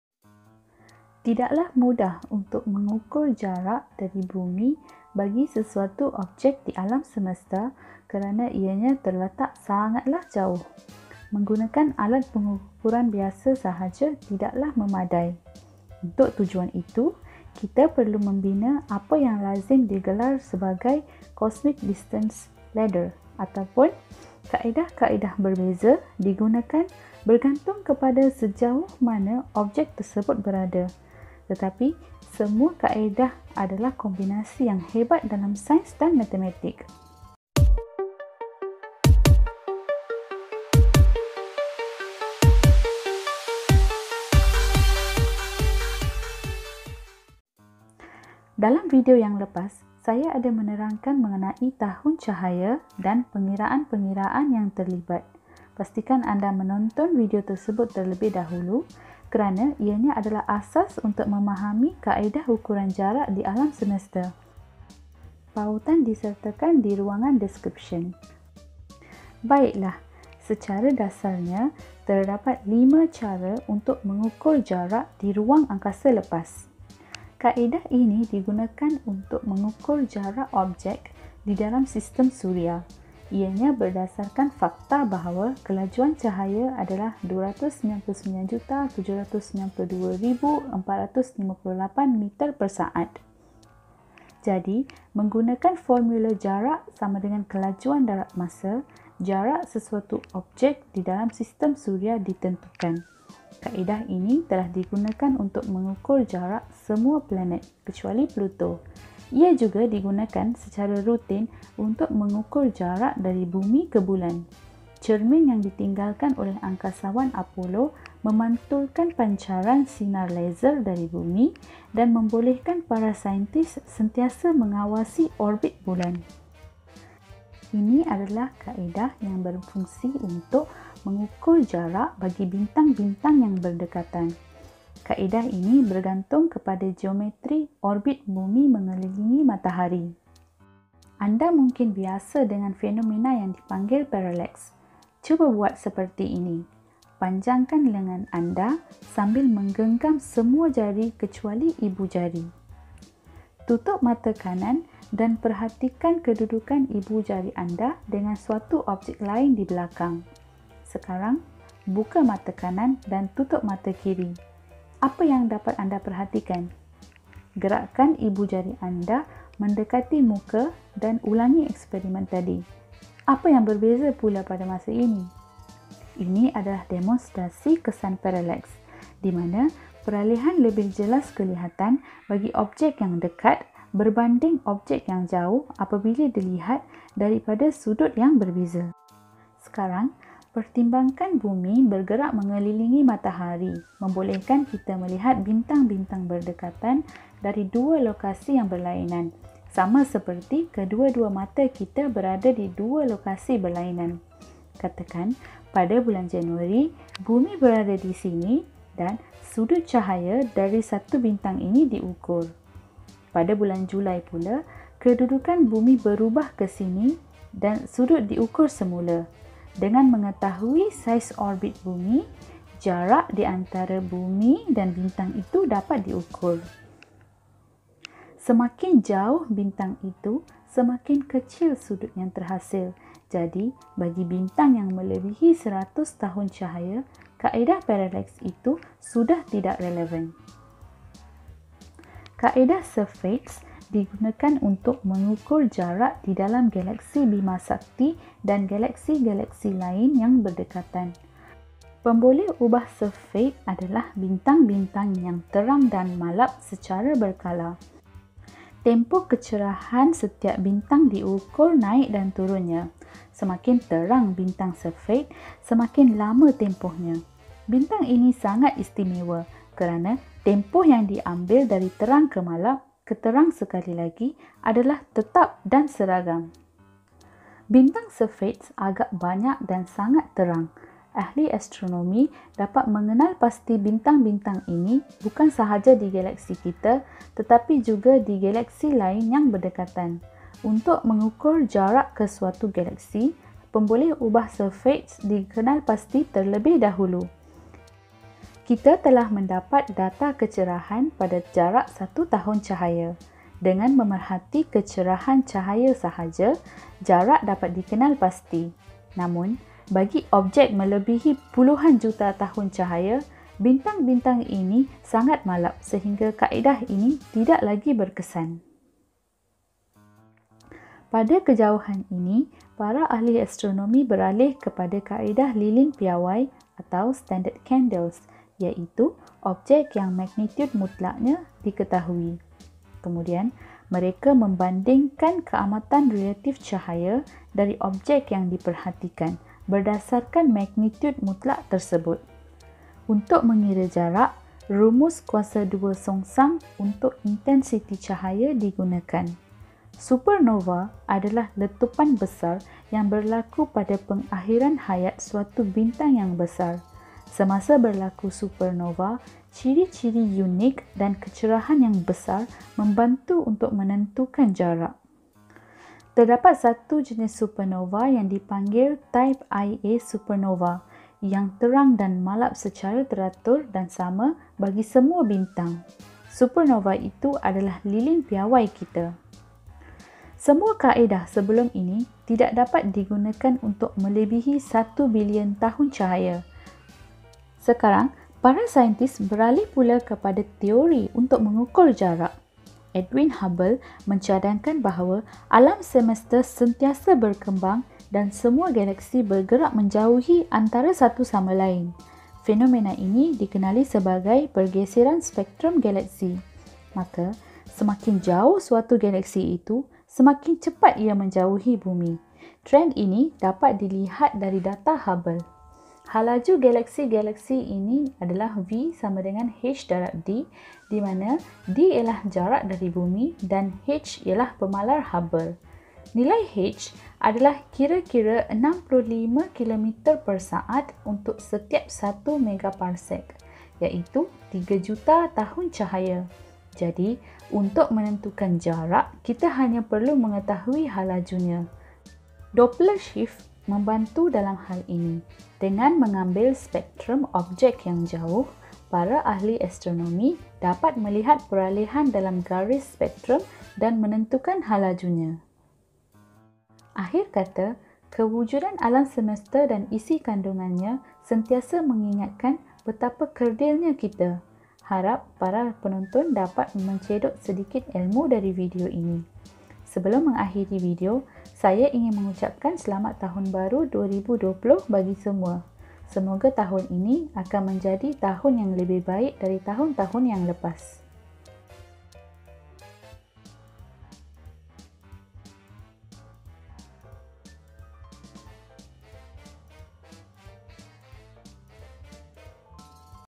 Alat pengukuran di angkasa adalah tangga? 🌌 | Bahagian II | Mari Belajar
Video ini mendedahkan secara ringkas 5 kaedah untuk menentukan jarak di ruang angkasa lepas.

Pembetulan: Sebutan bagi supernovae type Ia ialah 'supernova jenis satu A'.